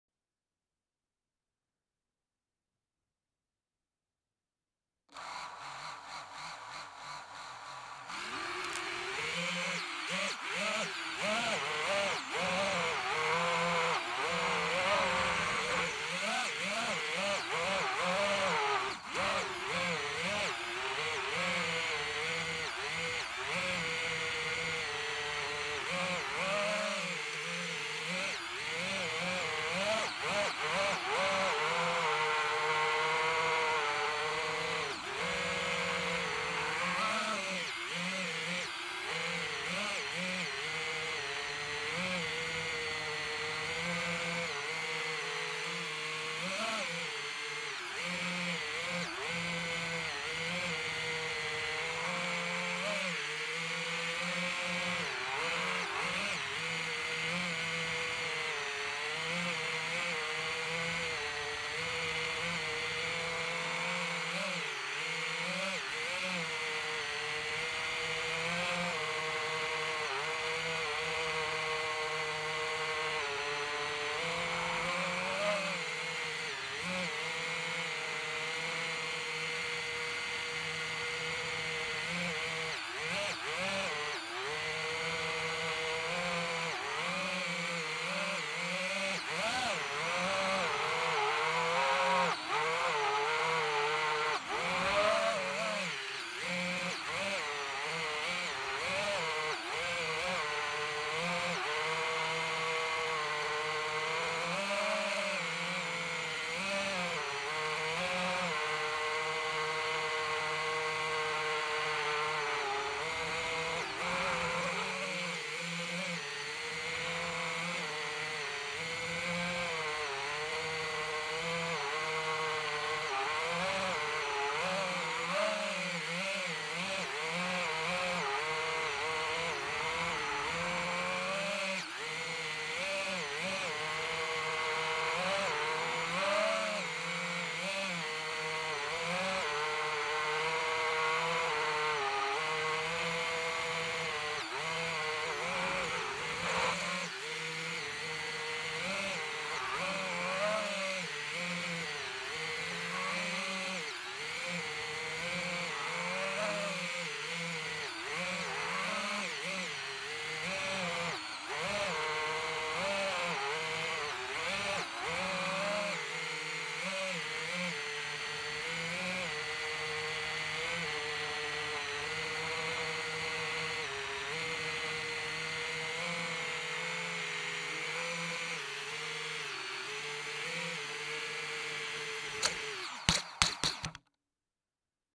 dji fpv test
부경대 뒷골목에서 ,. test 했습니다
역시 어두운곳 안보이는 가지 있는 장소는 dji fpv 정말 잘보입니다 ...
역시 good~!